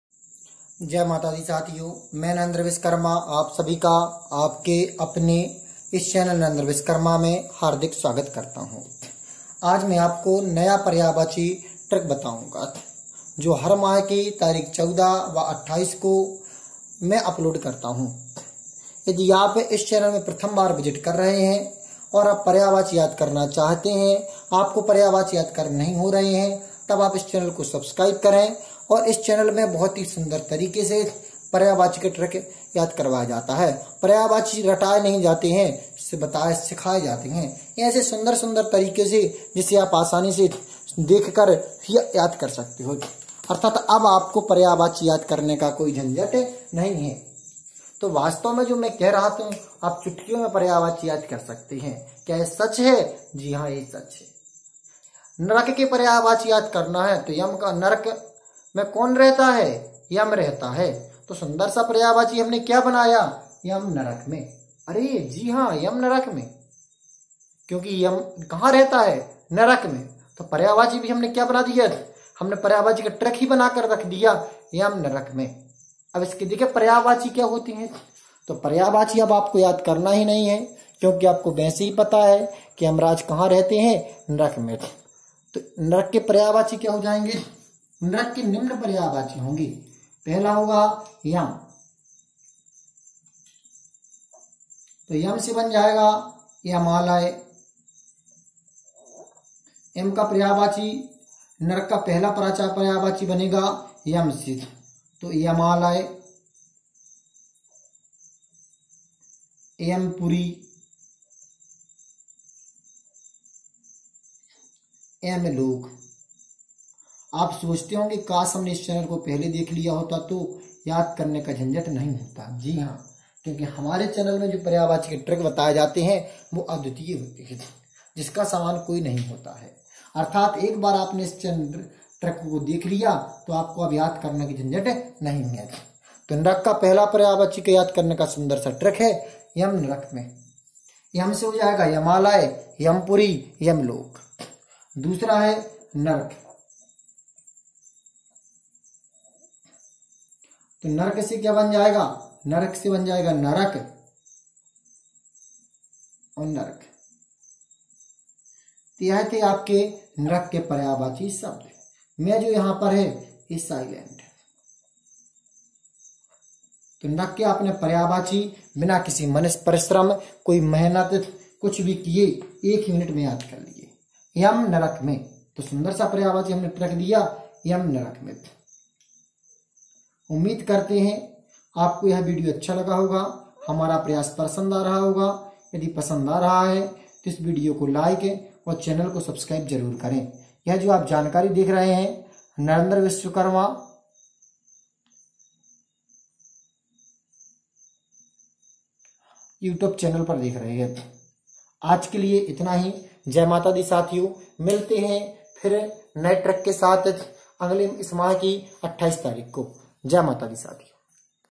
narak ka paryayvachi shabd | yampuri ke paryayvachi | नर्क का पर्यायवाची
narak ke paryayvachi shabd yad karne ka trick
मै आपको इस video मे नर्क के पर्याय वाची नाम याद करने का best trick वताऊँगा।

           🙏🙏धन्यवाद साथियो🙏🙏